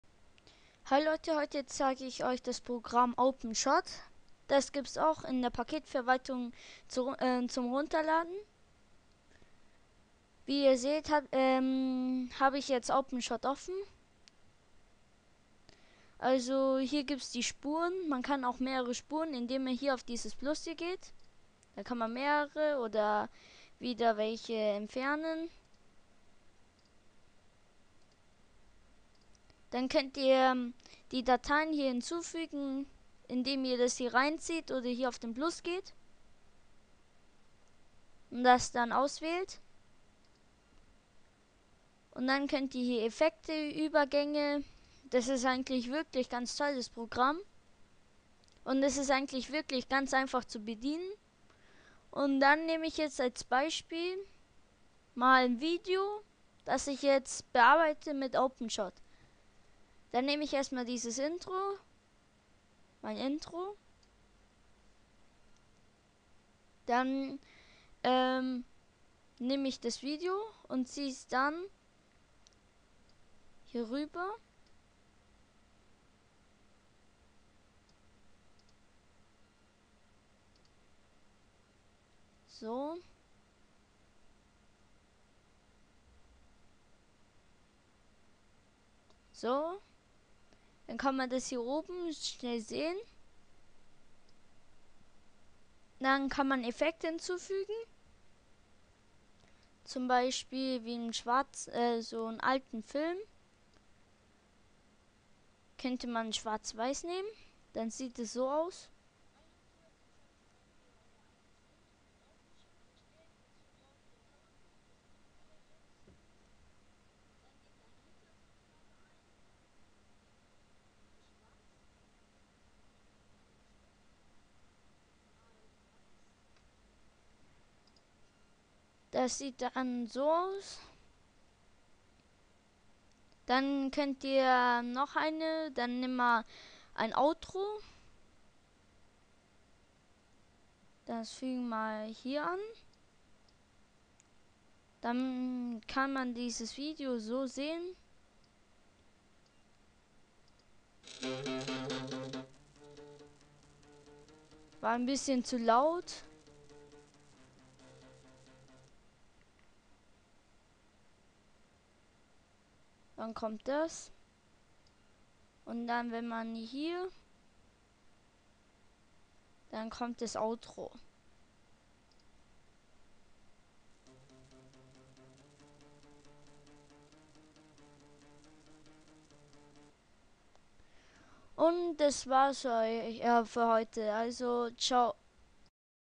Openshot Einführung
Heute zeig ich euch Openshot ein Videobearbeitungsprogramm.